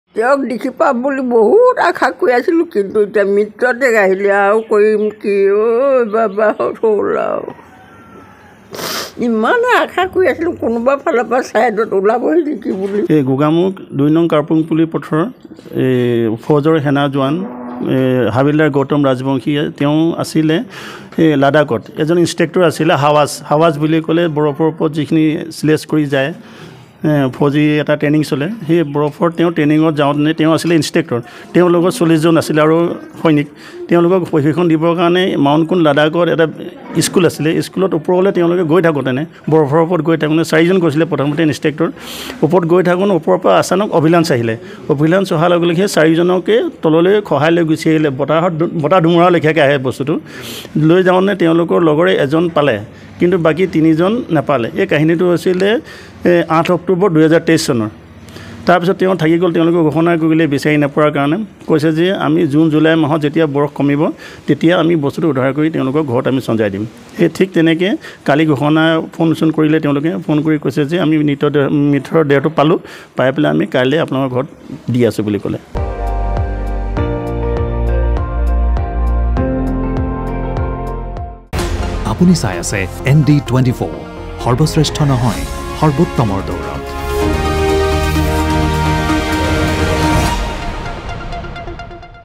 Assam News: লাডাখত উদ্ধাৰ গোগামুখৰ সেনাৰ হাবিলদাৰৰ মৃতদেহ
লাডাখত উদ্ধাৰ গোগামুখৰ সেনাৰ হাবিলদাৰৰ মৃতদেহ৷ মৃত হাবিলদাৰজন গোগামুখৰ গৌতম ৰাজবংশী৷ ১০ মাহ পূৰ্বে সন্ধানহীন হৈছিল লাডাখত৷ প্ৰশিক্ষণ দি থকা অৱস্থাত মৃত্যু হৈছিল হাবিলদাৰ গৌতমৰ৷

▶️ ৰাজপাট কাৰ?